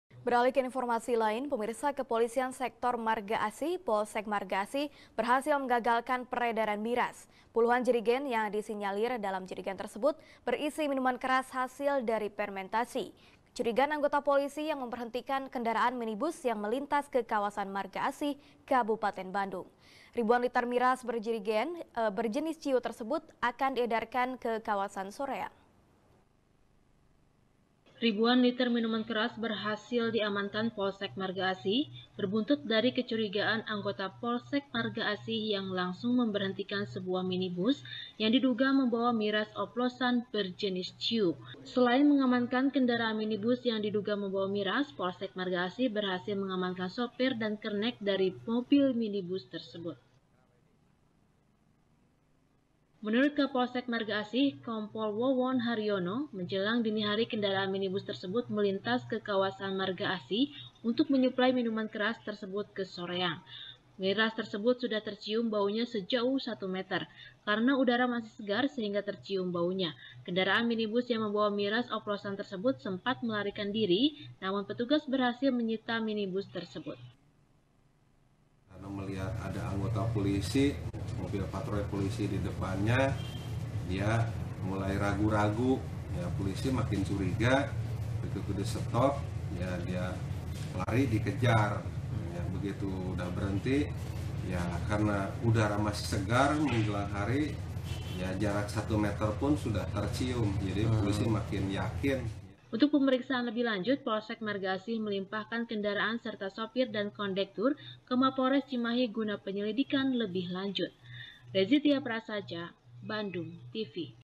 Polisi Gagalkan Peredaran Miras Jelang Ramadhan | SBR | BANDUNG TV
SBR | BANDUNG TV | DINDA RIZKYANA | Kepolisian sektor margaasih, polsek margaasih berhasil menggagalkan peredaran miras. puluhan jerigen yang disinyalir dalam jerigen tersebut berisi minuman keras hasil dari permentasi, kecurigaan anggota polisi yang memberhentikan kendaraan mini bus yang melintas ke kawasan margaasih, kabupaten bandung. ribuan liter miras berjenis ciu tersebut akan di edarkan ke kawasan soreang.